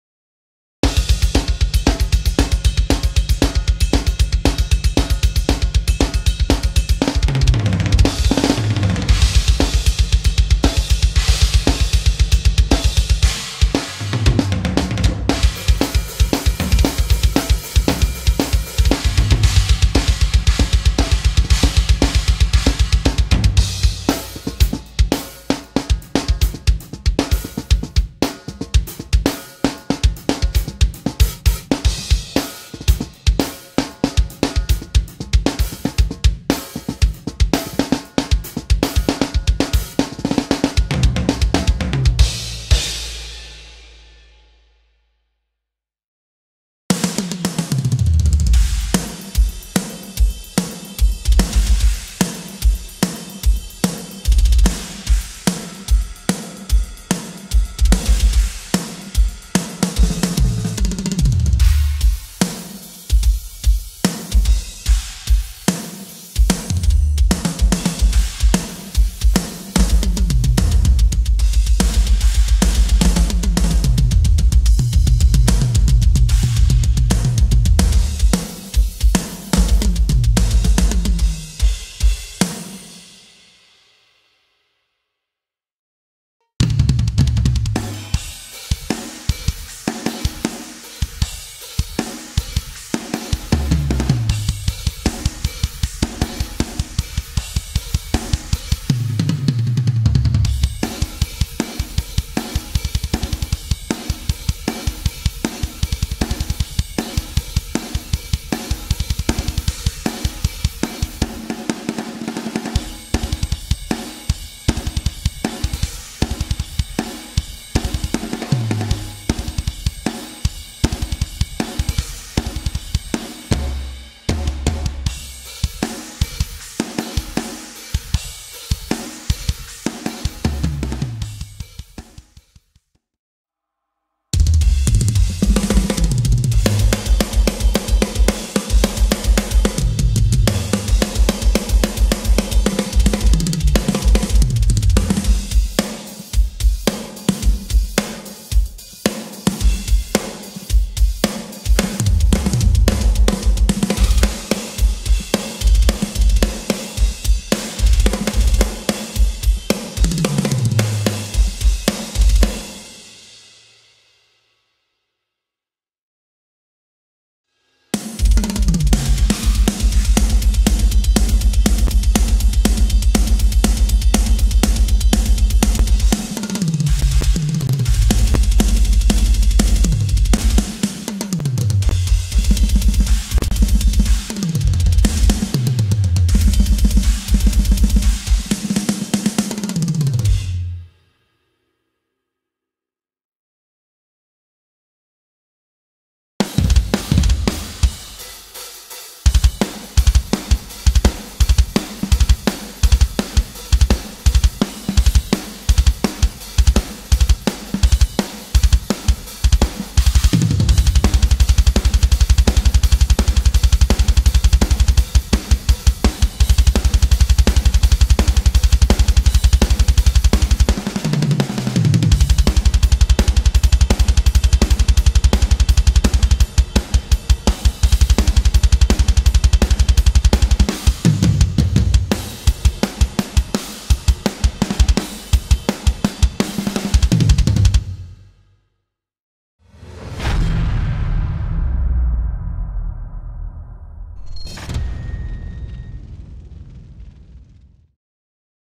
Superior Drummer 3 Custom Preset Compilation (With Free Download Links)
0:00- THE BLACKJACK

0:45- THE PATRIOT

1:26- THE KAISER

2:12- GOD OF FIRE

2:47- THE SENTRY

3:15- THE GOLDEN KING

-Toontrack's Superior Drummer 3 Free Custom Presets-